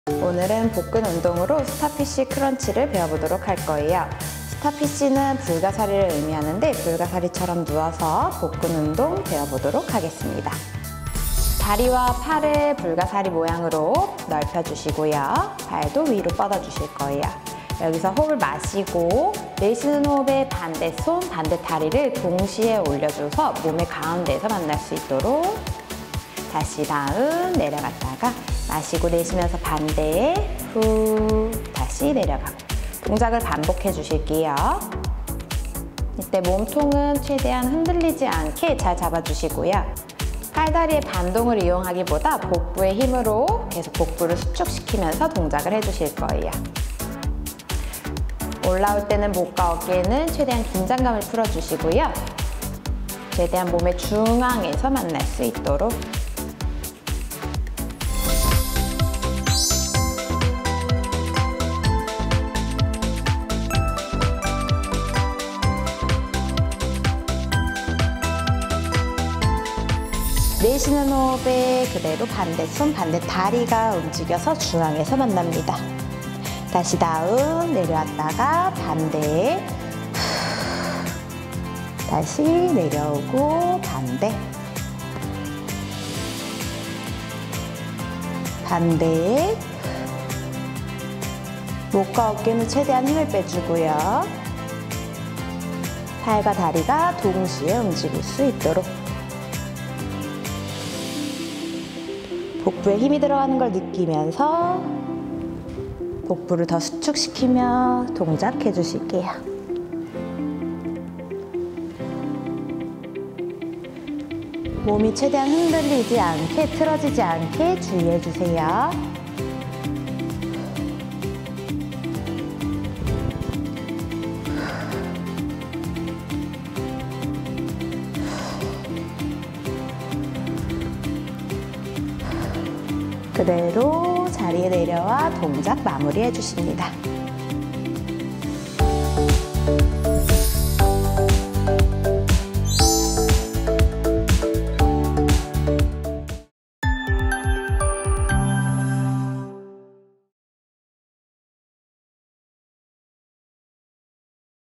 스타피쉬 크런치 / 예뻐지는 1분습관 - 1분홈트
오늘은 복근 운동으로 스타피쉬 크런치를 배워보겠습니다.
불가사리처럼 누워서 하는 복근 운동입니다.

월~금 오전 08:30 ~ 09:30